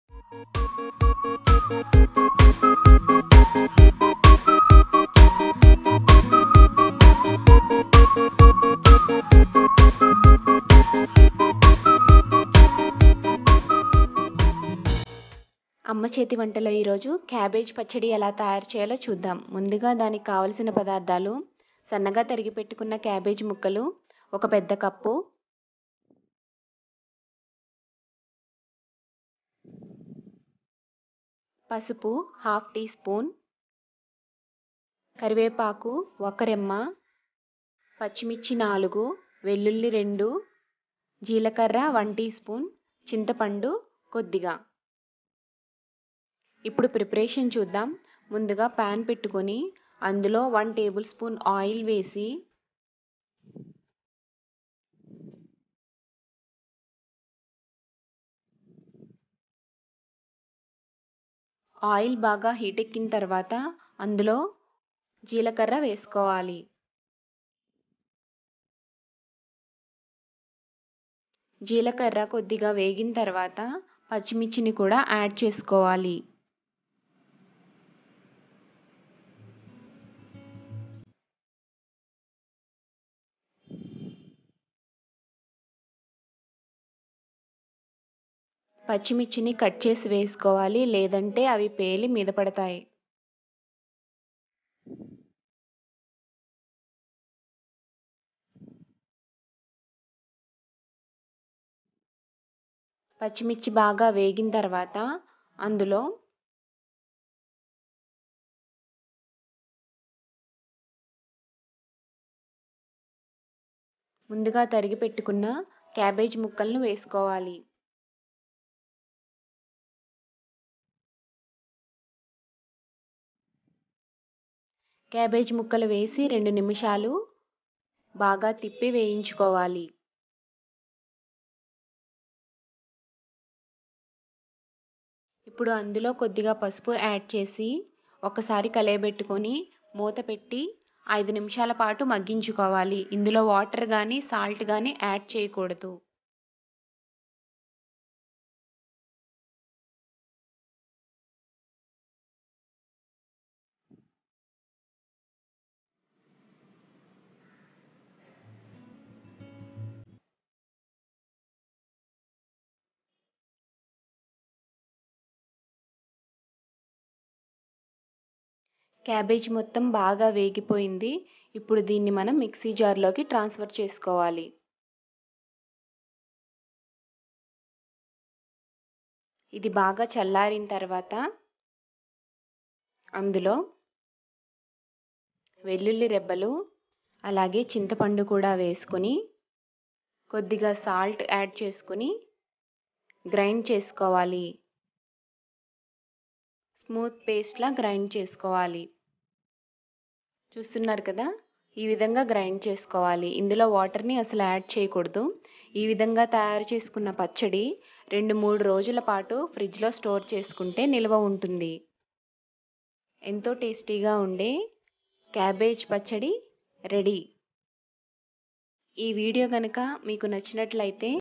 Cabbage Pachadi Recipe In Telugu | Cabbage Chutney Recipe Preparation By Amma Cheti Vanta
Spicy and tasty chutney for rice: cabbage pachadi..which include the benifits of weight loss,good eye sight, good for constipation too...